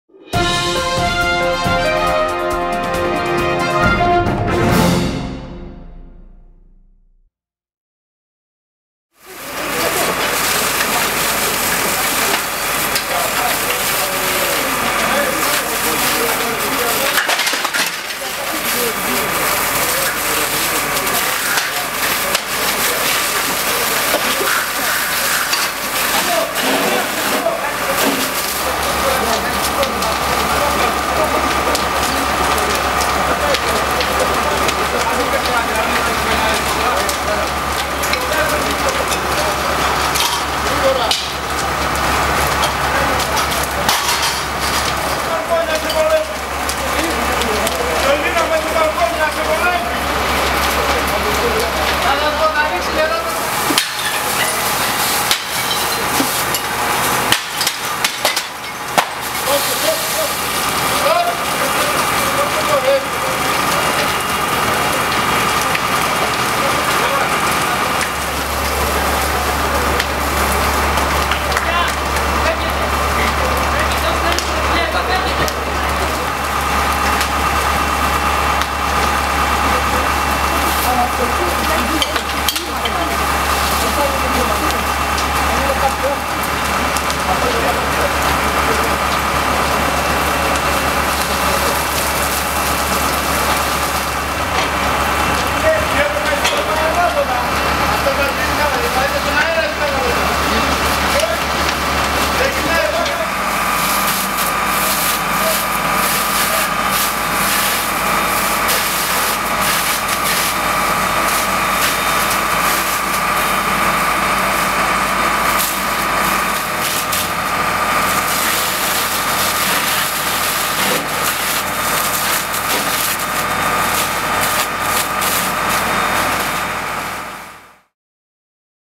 ilialive.gr - Φωτιά στο κέντρο της πόλης
ilialive.gr - Φωτιά στο κέντρο της πόλης - 08 Μαρ 2015 - Φωτιά ξέσπασε σε εγκαταλελειμμένο διώροφο κτίριο στο κέντρο της πόλης κοντά στο "Σταυροπάζαρο"